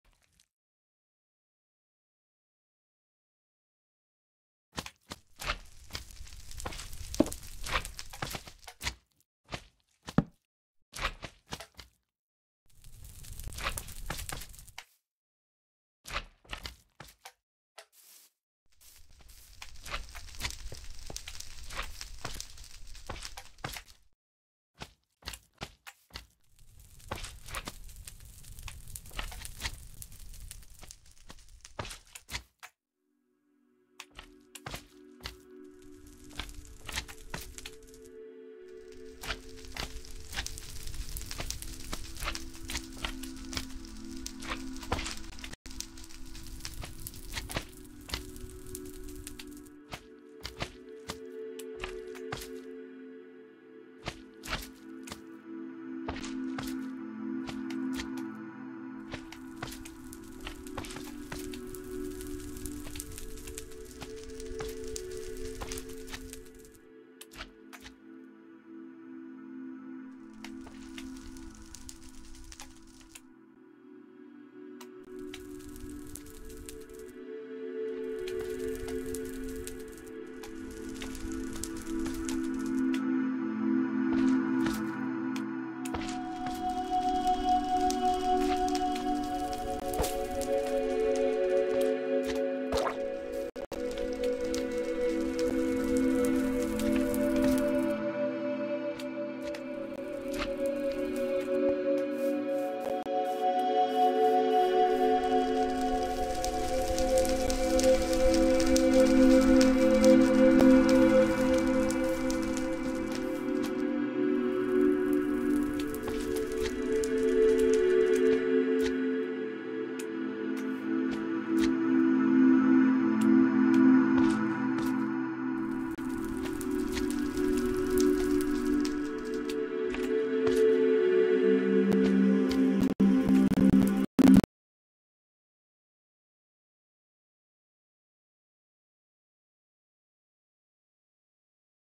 Día 119: poniendo bloques de terracota azul hasta tener una MDJ
Esta máquina dj es algo que me inventé en mi esquizofrenia de tanto ver Apixelados jaja